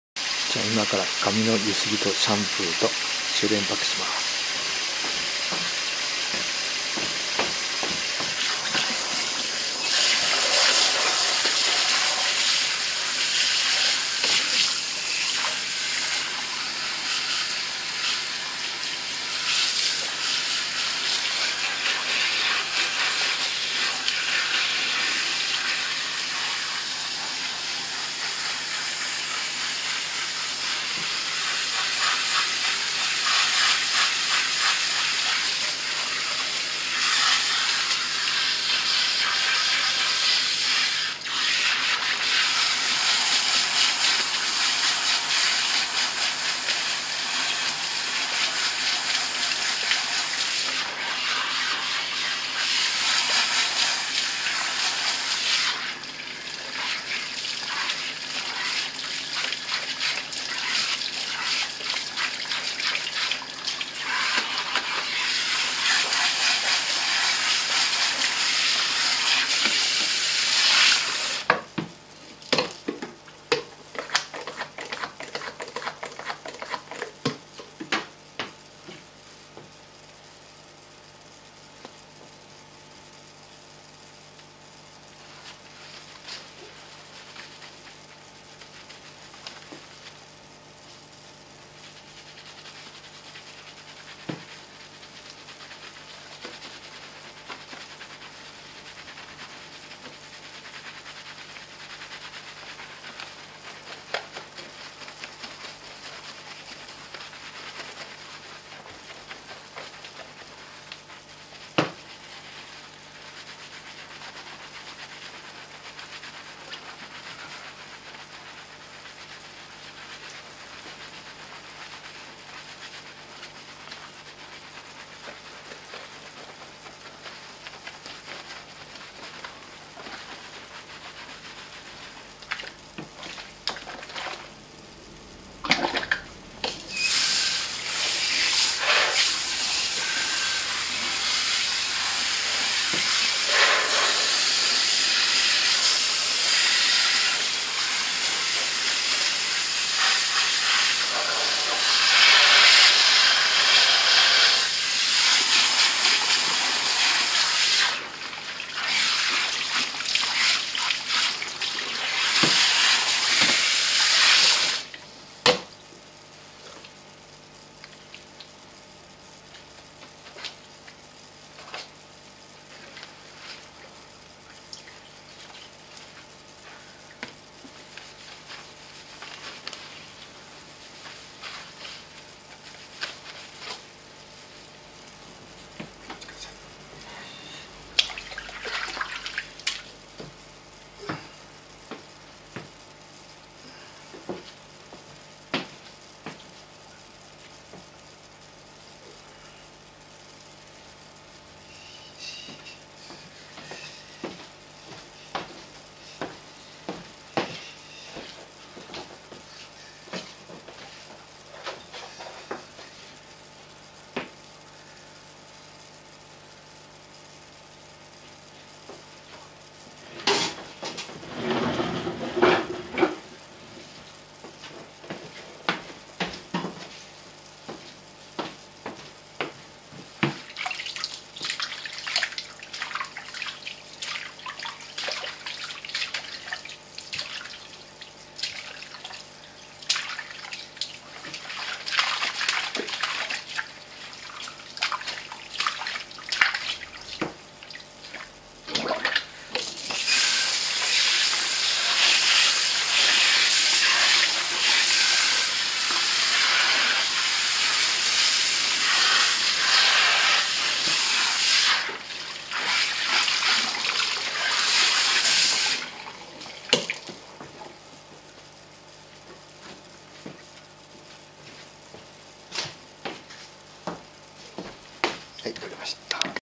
美容室あ～む 段差の多くあるカットとイチゴカラー白髪染め 渋川市
URLarm2006.com   　　髪のゆすぎとシャンプーと収斂パックをします。